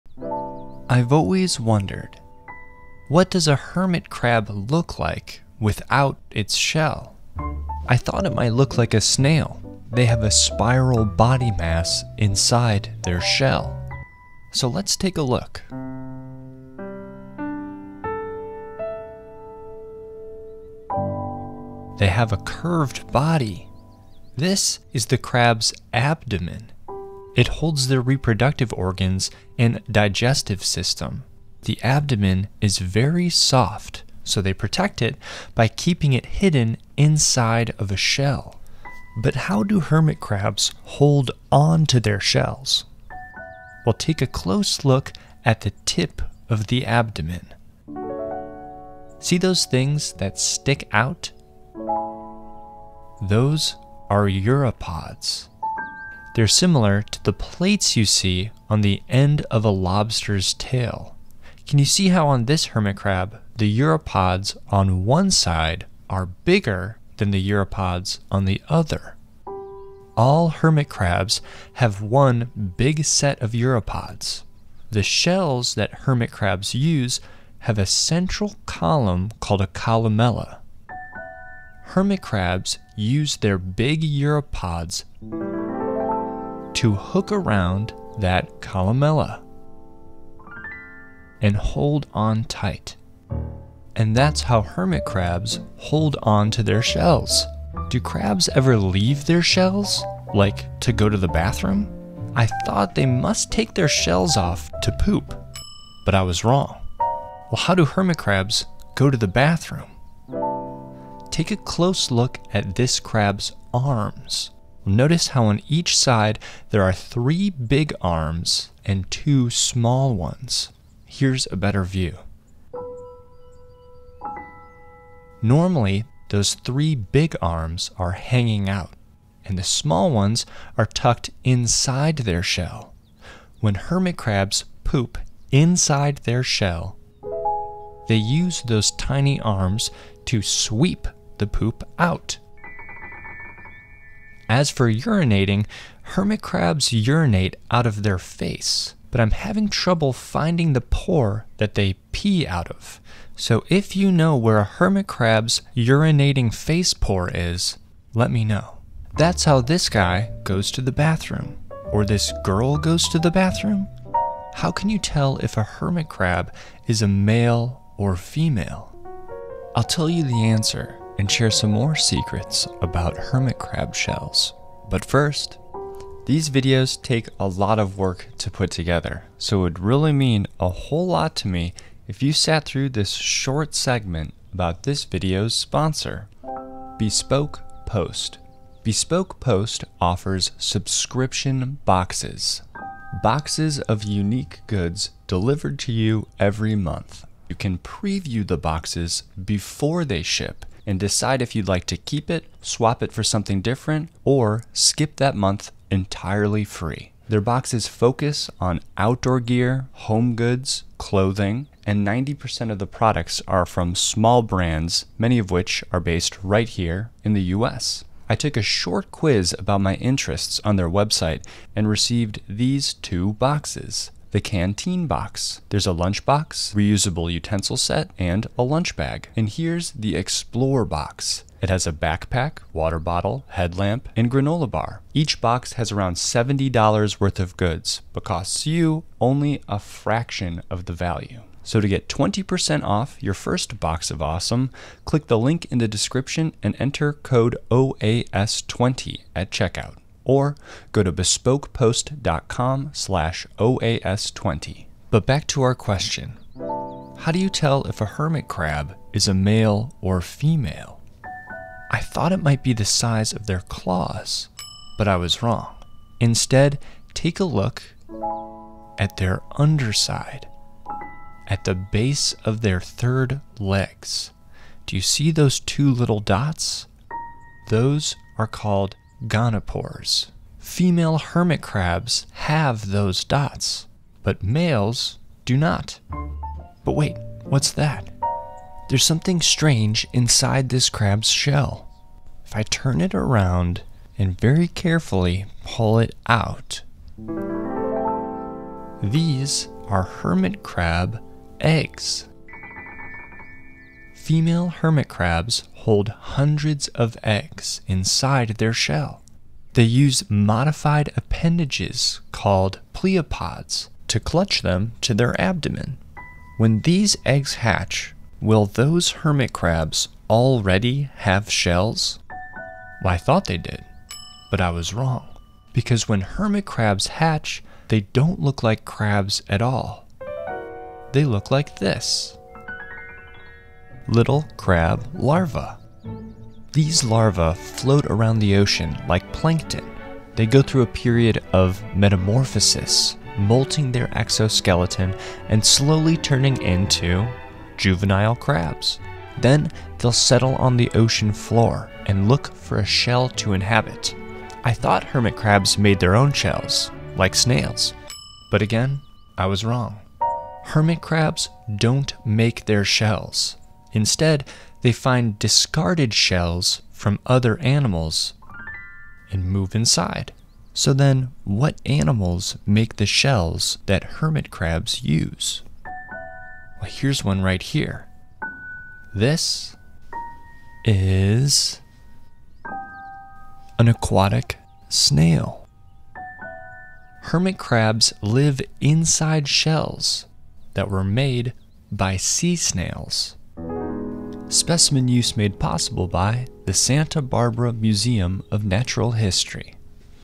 What's Inside a Hermit Crab Shell?
This video is about hermit crabs. It discusses what hermit crabs look like without their shells and can help answer the following questions:

- What does a hermit crab look like without its shell?
- Do hermit crabs poop inside their shells?
- How many legs do hermit crabs have?
- When do hermit crabs get their shells?
- Are hermit crabs born with their shells?
- How to tell the difference between a male and female hermit crab?
- Do hermit crabs lay eggs?
- What animal makes hermit crab shells?
- Do hermit crabs make their own shells?
- Do hermit crabs ever leave their shells?